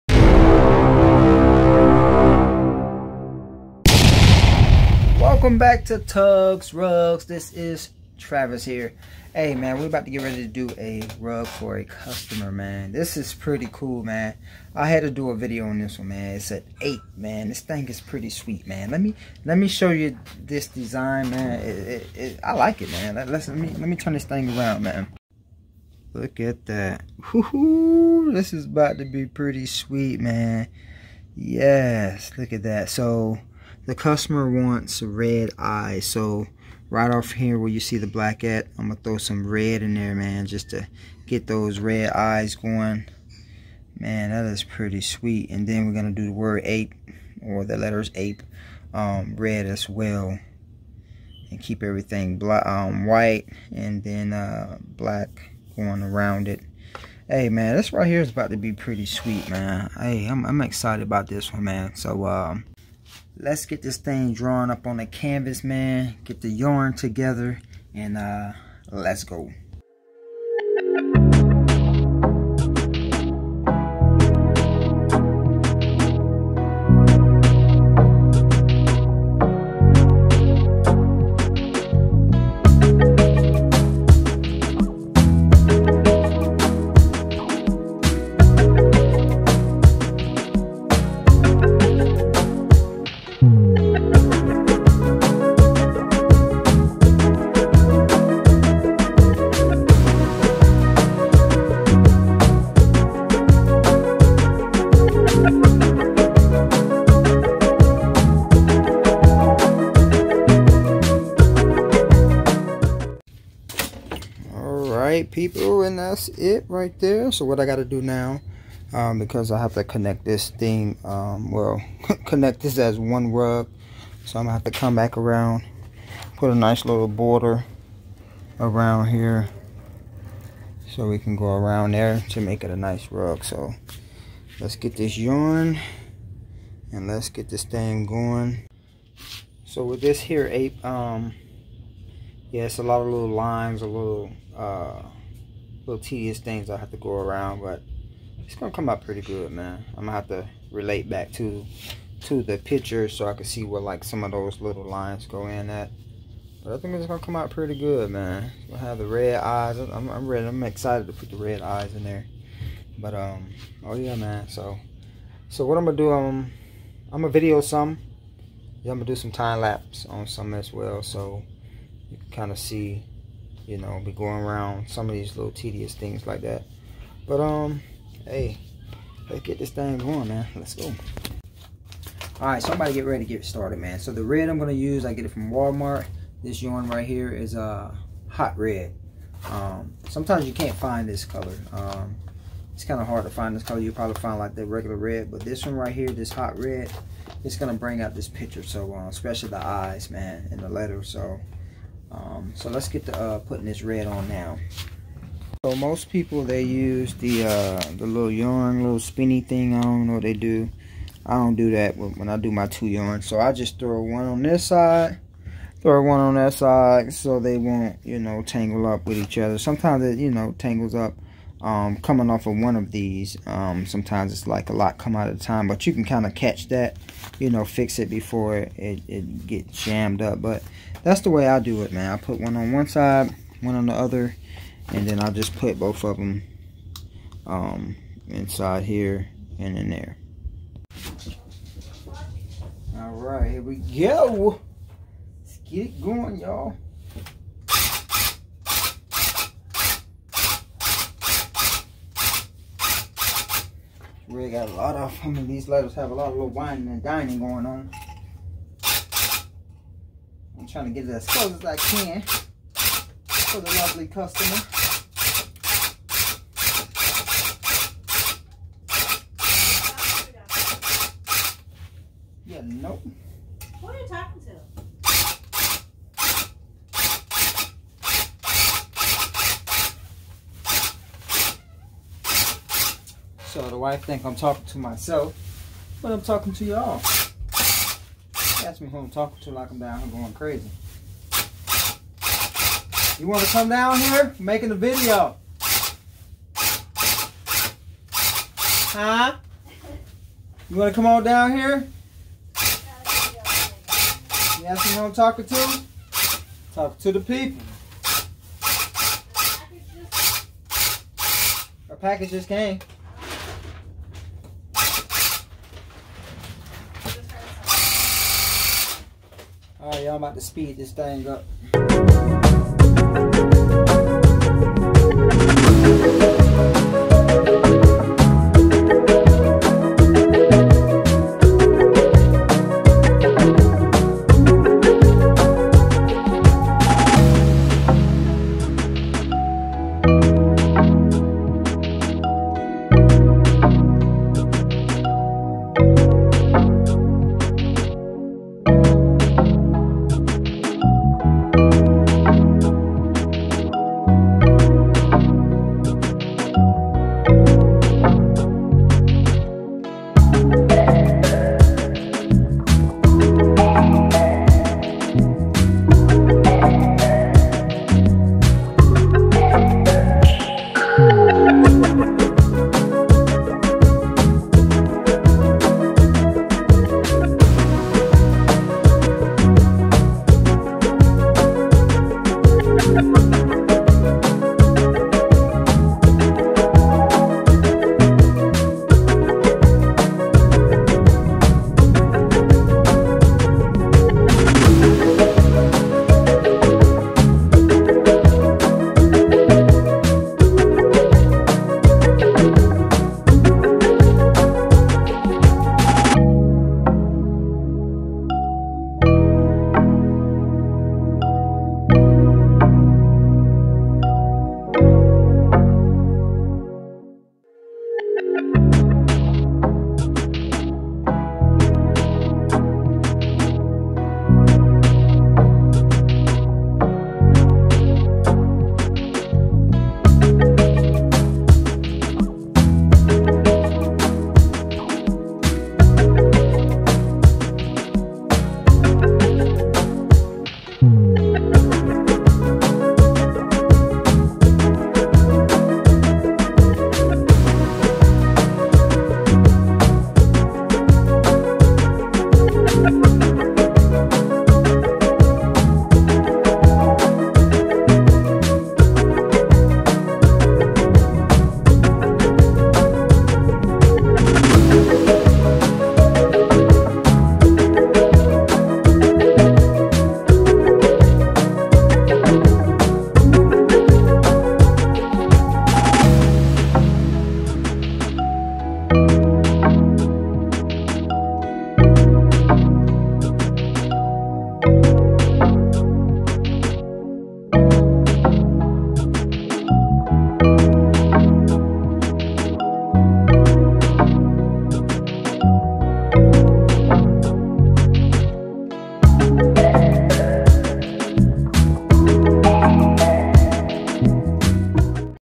Tufting an APE Rug Tugs Rugs
Custom Rug Tufting. Materials are below

Tugs Rugs Materials Used

All yarn bought from Walmart or Joanne's Fabric
Walmart The Cheapest

Cut Pile Tufting Guns

Electric Stapler

Burlap Fabric:
(60inch x 100 yards)

(60inch x 20 yards)

(40inch x 50yards)

Sheep Shears

Hot Glue Gun:

Carpet Glue:

Adhesive Spray: